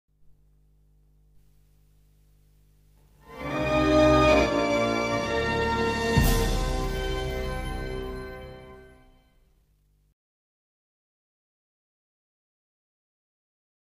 Royal Oaks home entertainment logo (2002)
this is the remake of this logo i made, because I feel like it,

well, I fixed the green tree for blurry vision, so I put the AOL time warner byline on bottom, and make it look like it's royal Oaks home entertainment

for it's DVDs, the logos are, Dolby Digital rain logo (1998) and, Macrovision quality protection logo (1997)
(closing Only)